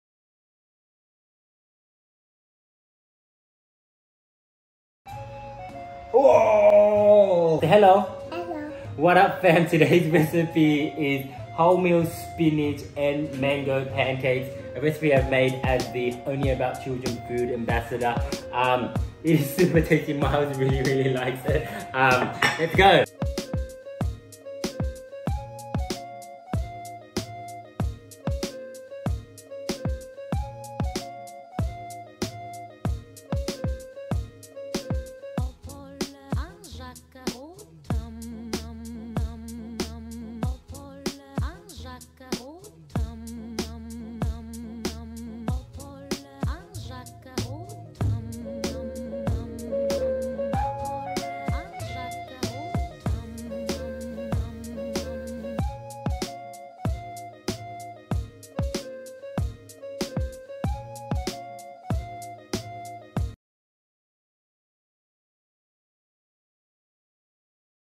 Only About Children - Grow With Us - MasterChef Tommy Pham's Wholemeal, Spinach & Mango Pancakes
Watch Only About Children Food Ambassador, MasterChef Tommy Pham, make Wholemeal, Spinach & Mango Pancakes to celebrate National Pancake Day!